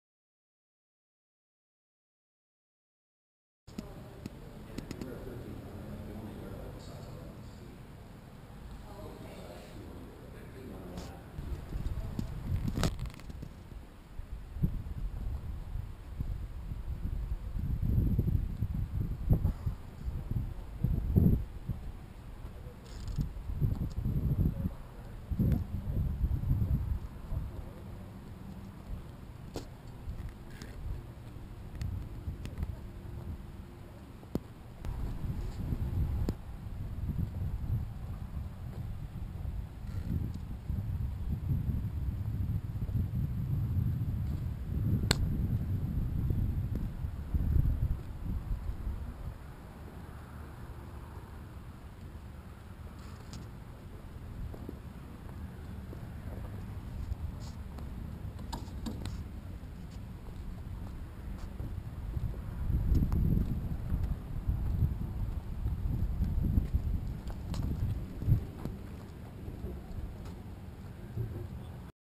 Clavet Fire 2015 Open House Tour
Quick tour of the Clavet Fire Rescue equipment.

Clavet Fire Rescue Open House on Oct 10, 2015 to kick off Fire Prevention Week.